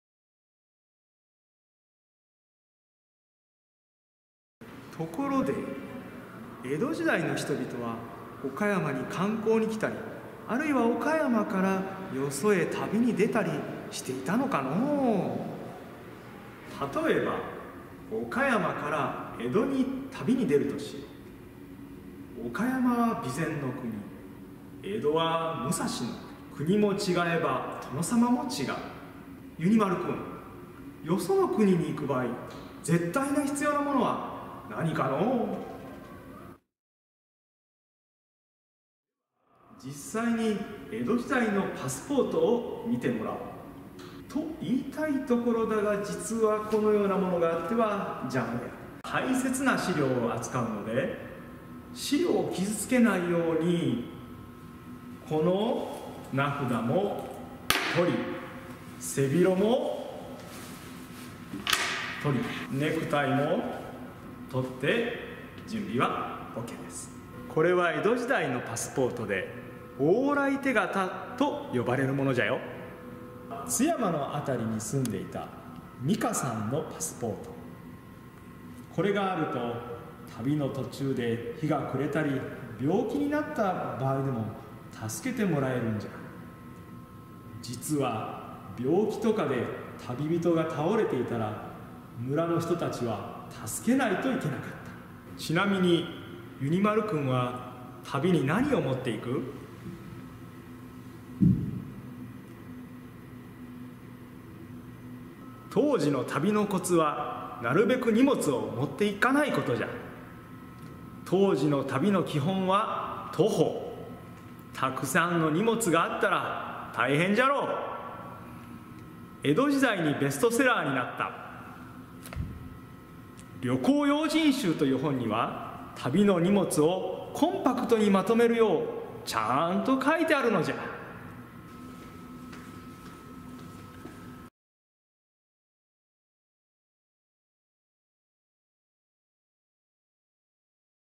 県立博物館②～江戸の旅パスポート編～
きっず☆ユニバon-line。岡山県立博物館で江戸時代の旅をちょっと、体験してみよう。